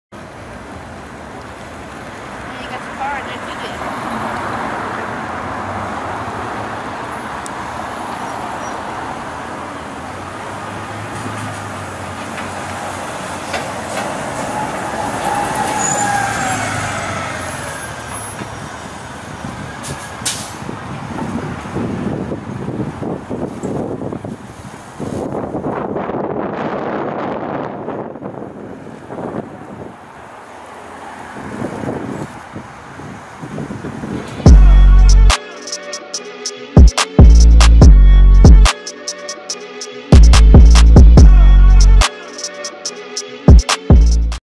New Jersey Transit 2014-2015 MCI D4500CT CNG #7280 on the 64H to Lakewood
64H to Lakewood via route 9 local in Jersey City along Christopher Columbus Dr